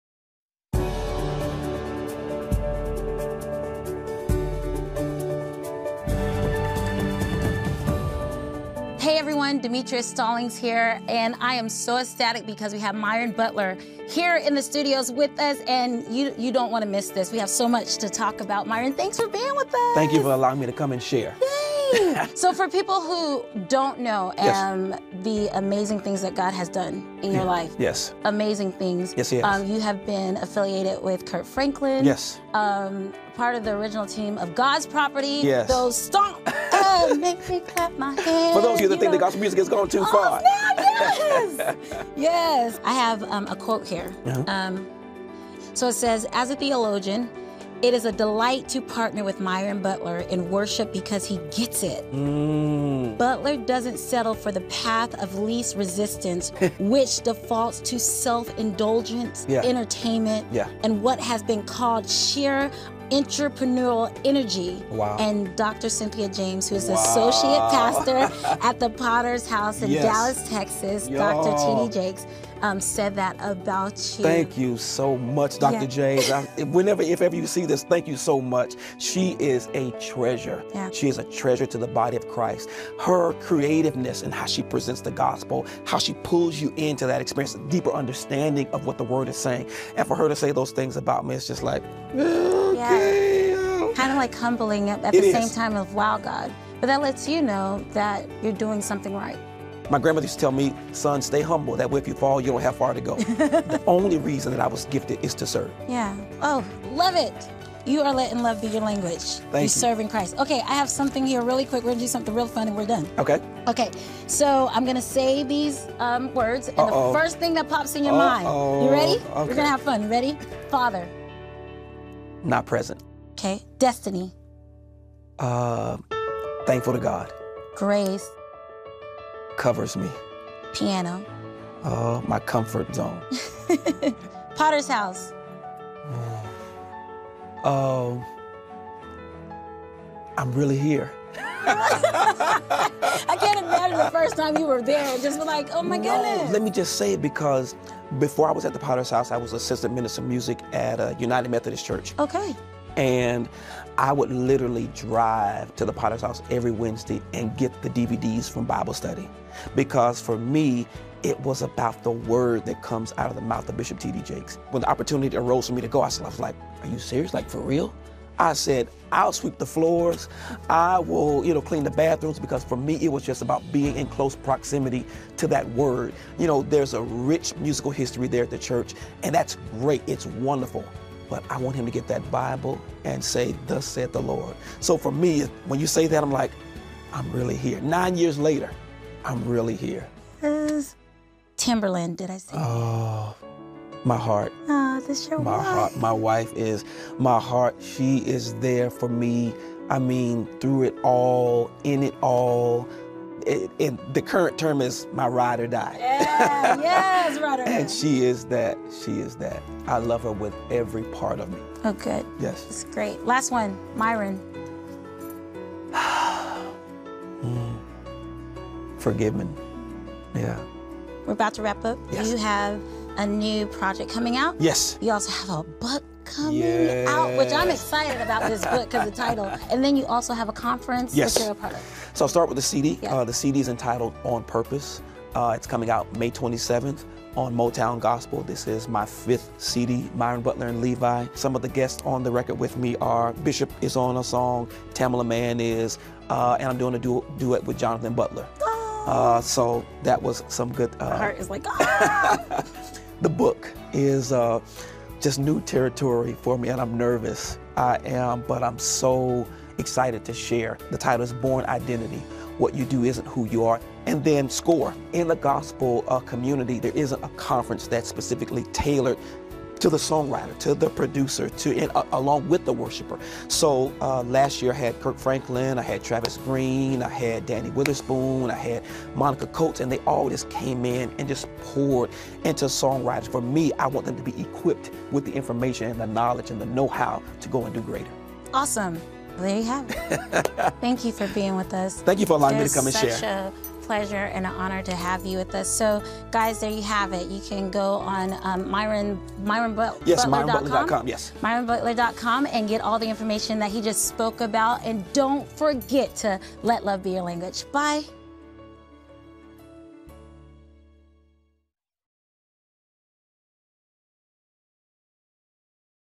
Myron Butler: Web Exclusive
Grammy Award winner; Myron Butler, shares his heart for music, worship, and his experience as a worship minister at The Potter’s House under Bishop T.D. Jakes